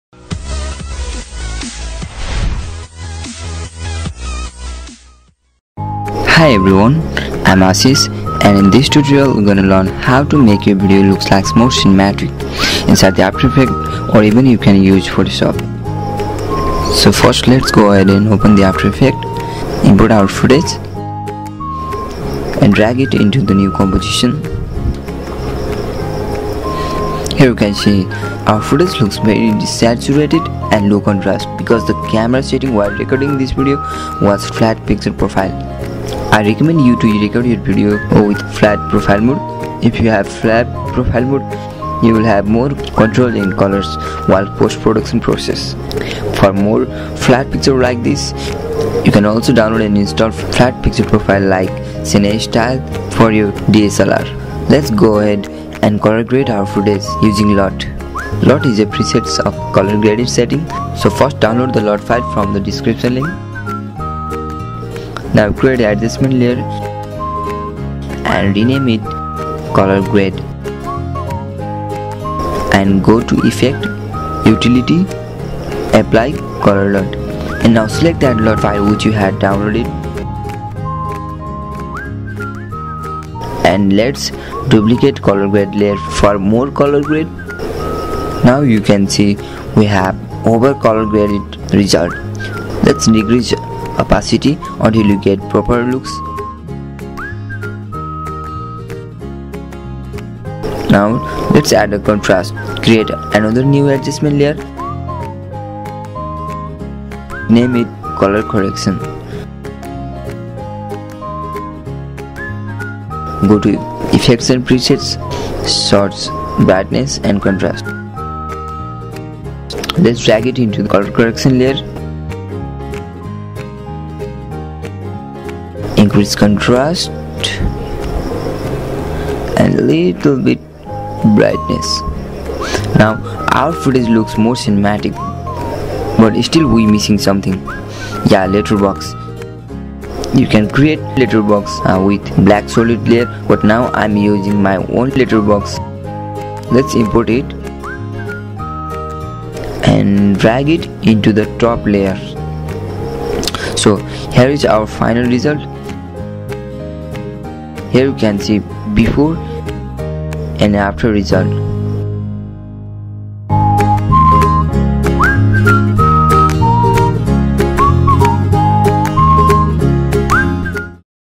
Cinematic Looks in After Effect Using LUT
Hi everyone! Today we're learning a quickly little technique on how to Color Grade videos and photos using LUT. It's actually pretty easy to do and you can be pretty creative/clever with it! I hope you guys enjoy the video and get to thinking on how YOU can start using this in your own work!